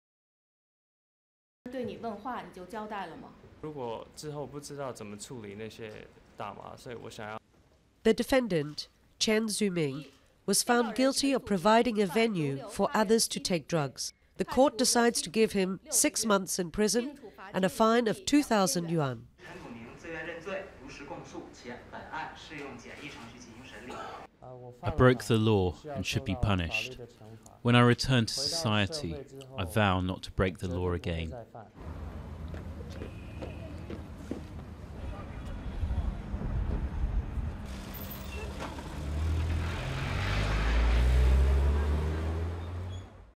Jackie Chan's son gets 6 months for China drugs offence: court
The son of kung fu star Jackie Chan admits drugs offences and is sentenced to six months in prison, a Beijing court says. Duration: 00:43